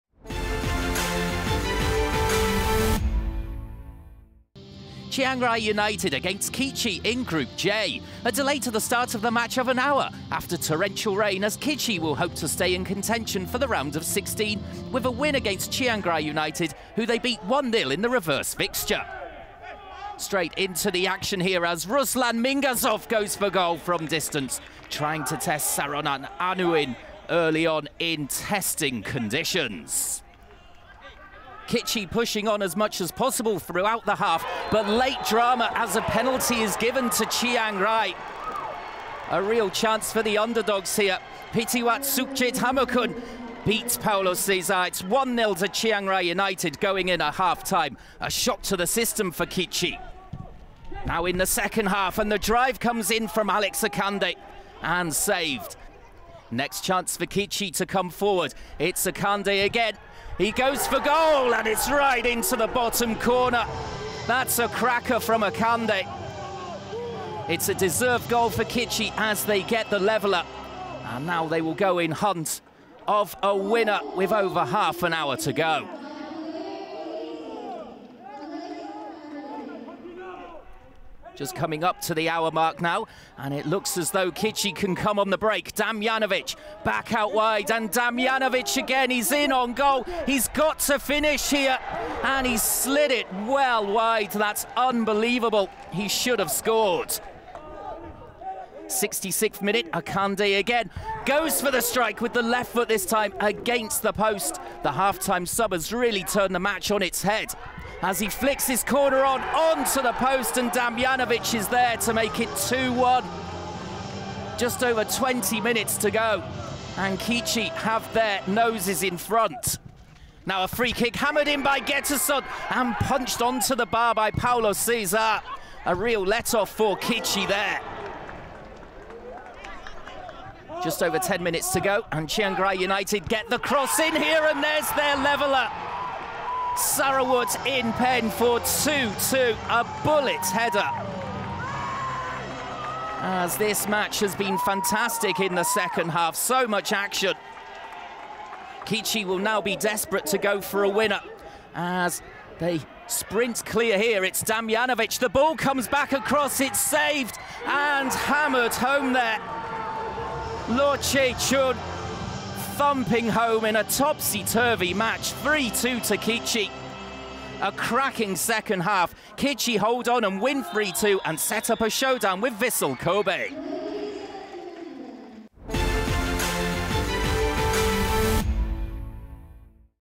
【亞冠盃-精華片段🌏🏆⚽ 】​2022-04-28 清萊聯🇹🇭 2:3 傑志 🇭🇰 | 傑志逆轉清萊聯全取三分
喺亞冠盃第五輪J組比賽入面, 傑志 🇭🇰嘅德揚建功，羅梓駿鎖定勝局, 最終傑志逆轉 清萊聯 🇹🇭全取三分👏, 依加一齊睇黎下呢場比賽嘅精彩片段​🎬
M直播係#亞冠盃 2022至2024 澳門區獨家轉播商​
🔥亞洲頂級賽事盡在M直播, 立即下載📲 ​